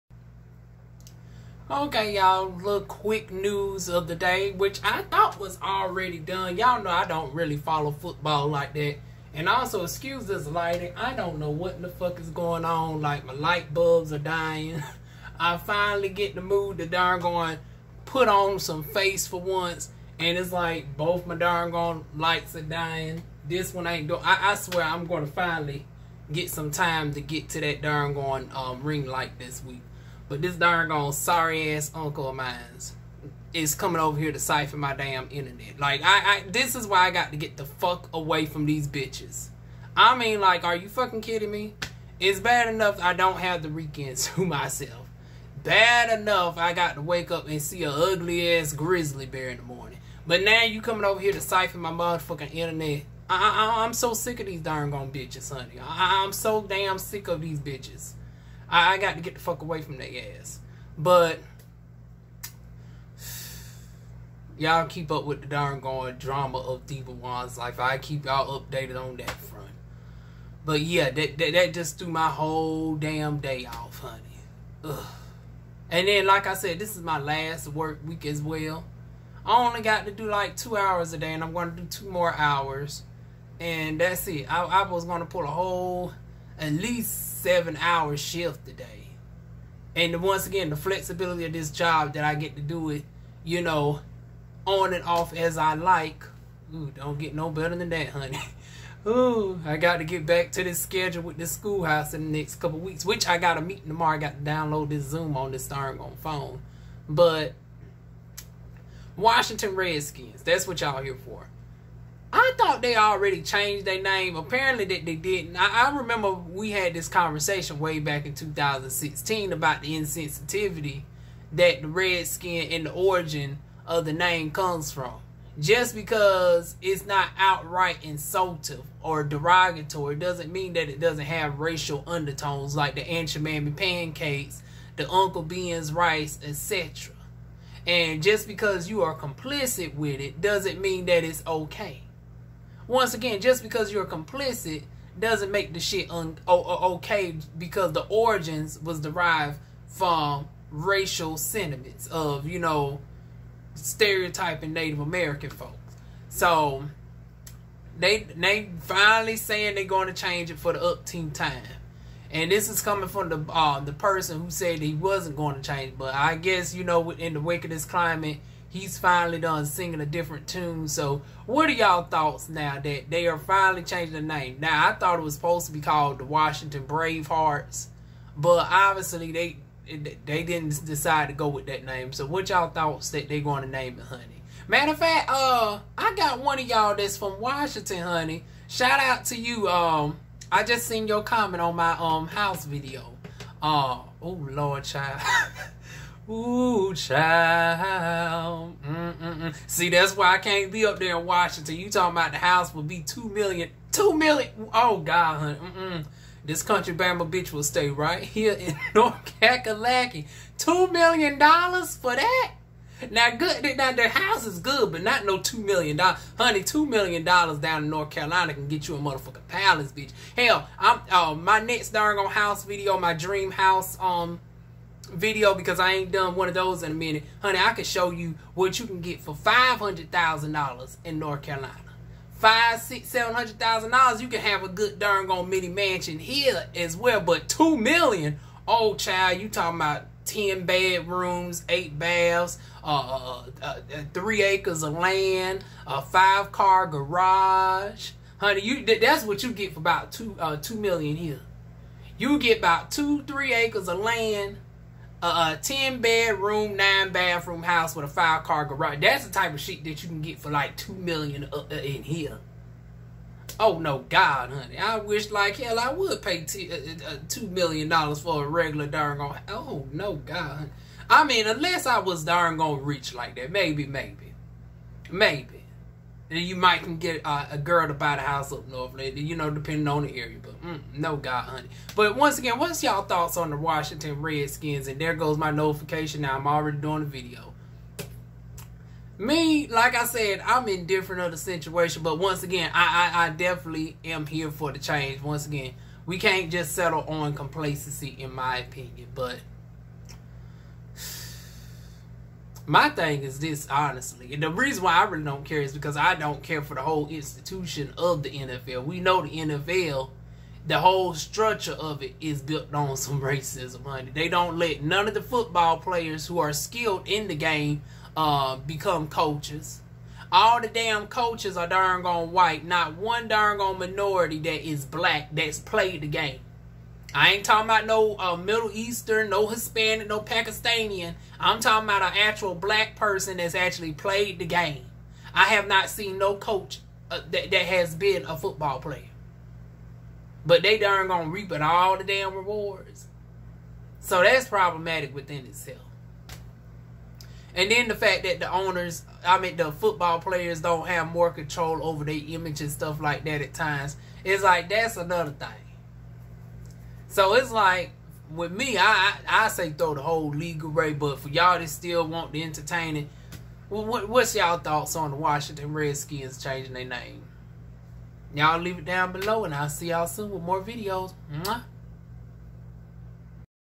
My Quick Thoughts On Washington Redskins Finally Changing Their Name
Just a quick video giving my thoughts on the football team Washington Redskins changing their football name due to their original name being racially insensitive to Native Americans.